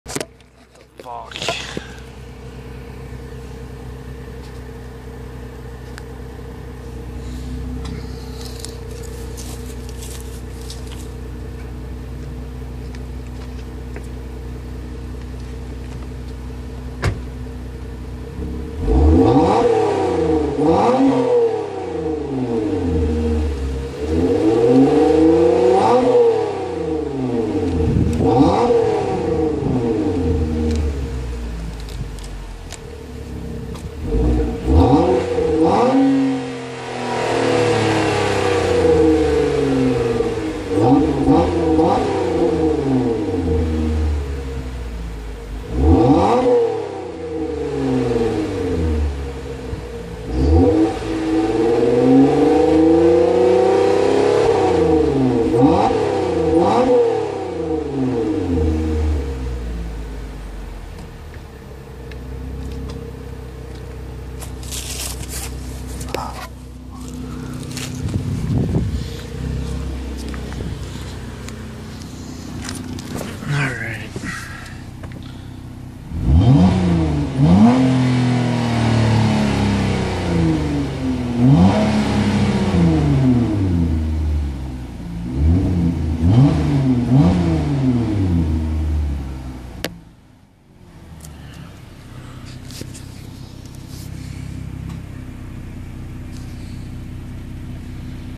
Flowmaster 10 with resonator added
Have resonator off look at my other vids was to loud so put it back